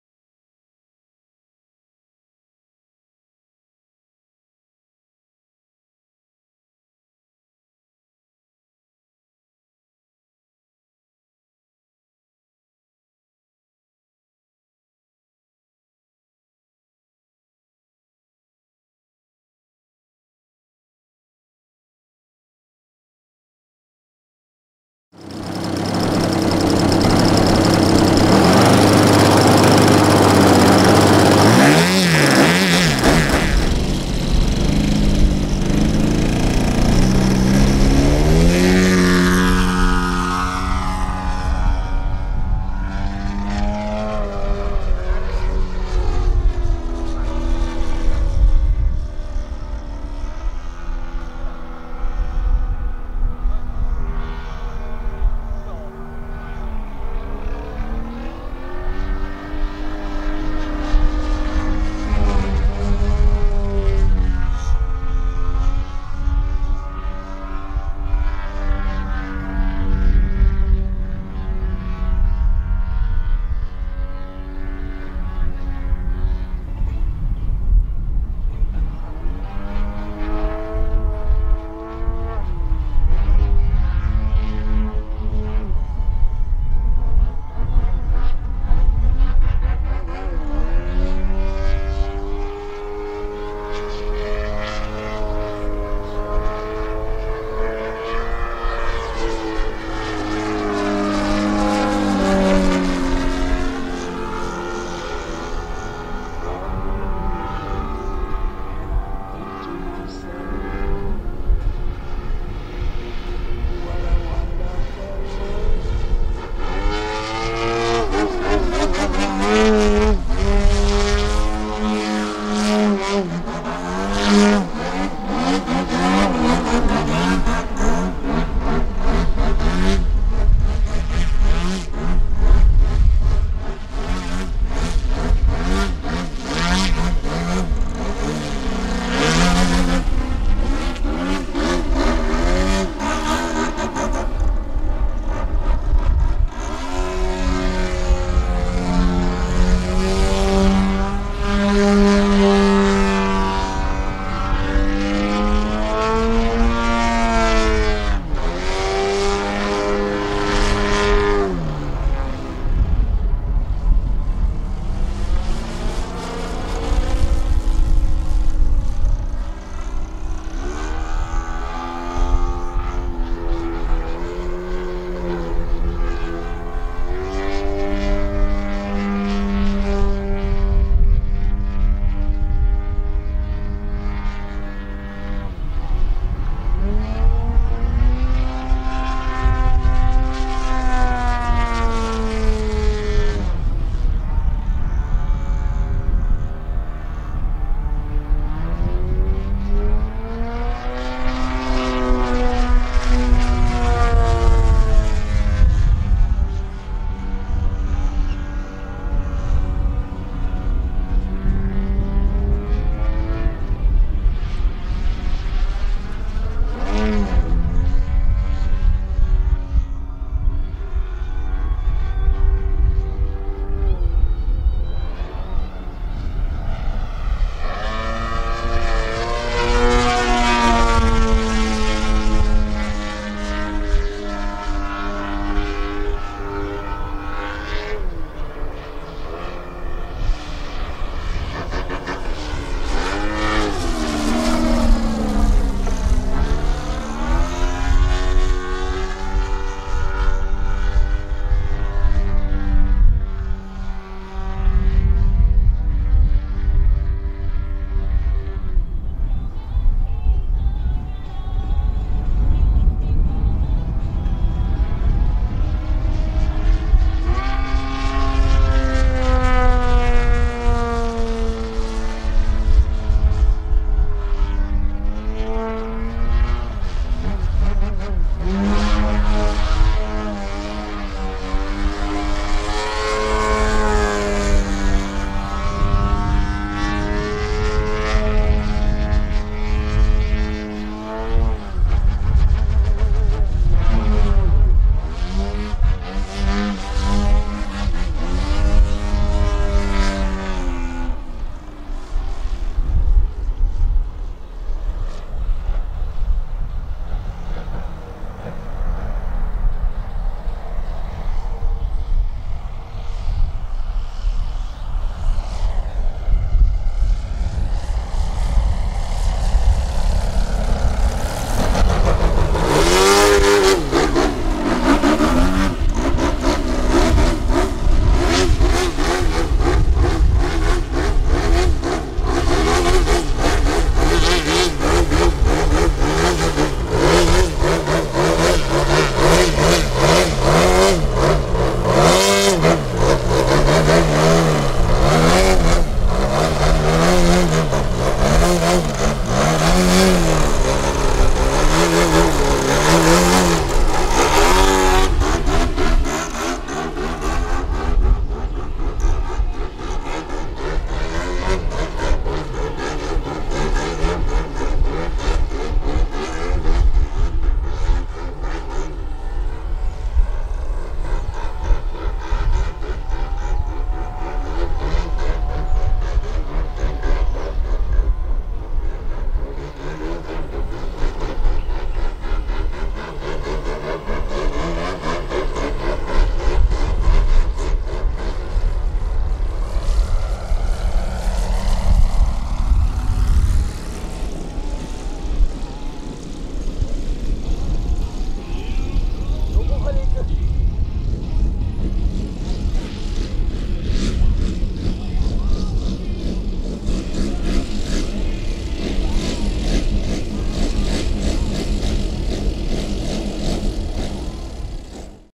大型ガソリン機 の曲技フライト @ 東庄ラジコンクラブ 2016/10/30
Joe君がエクストラ３００をフライトです。
動画掲載は保護者の了承を得ています。